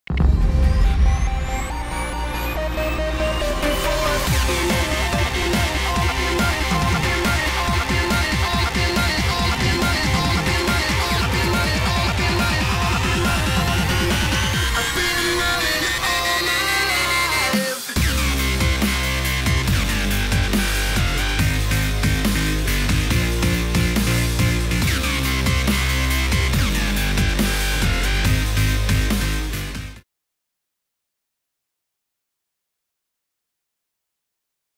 Boatside Chaos While Muskie Fishing - Version 2
In this video you will see World Champion muskie angler and guide Josh Borovsky demonstrate how to catch a muskie that follows to the boat on a very high speed retrieve.

"I prefer the figure 8 in most situations, however there is a time and place to opt out of the 8 based on how the fish is responding and where you are at in the progression of the 8" said Borovsky. "This catch demonstrates that well"

If you would like to learn more about fishing for muskies or experience a guided big fish trip,